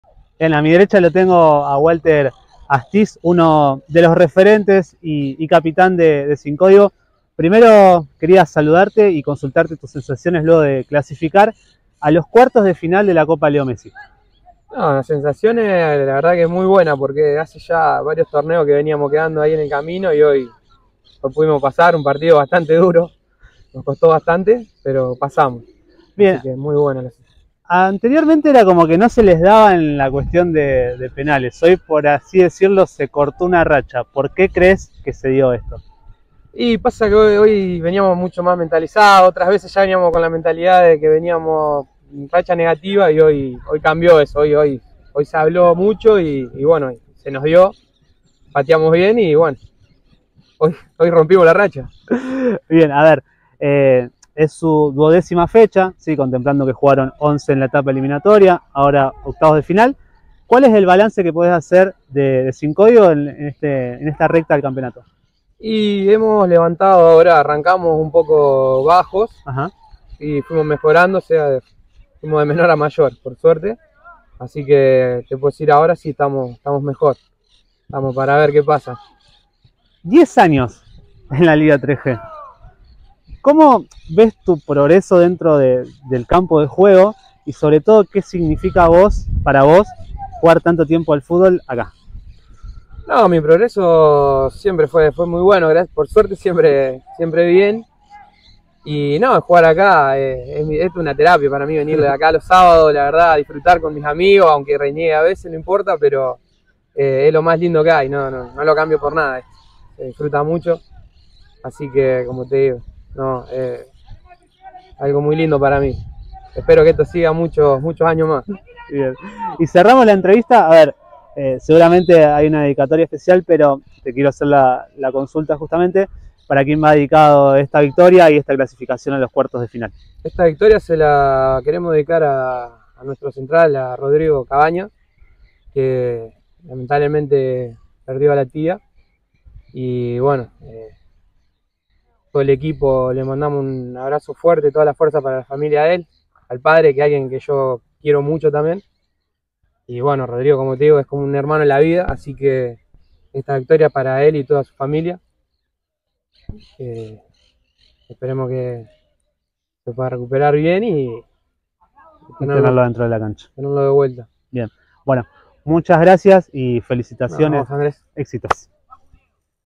Liga 3G – Nota a Walter Astiz (Sin Códigos)
Walter Astiz, uno de los jugadores que vienen disputando la Liga3G desde el 2013 con SinCódigos, habló con nosotros post victoria vs. Ambidiestros. El “Patrón” hizo un análisis del encuentro, del recorrido de su equipo en la CopaLeoMessi, de su década en la Liga 3G, y le brindó una dedicatoria al “Chori” Cabaña (¡fuerzas!), compañeros desde hace 10 años.

Auspicia Atocha FIT WEAR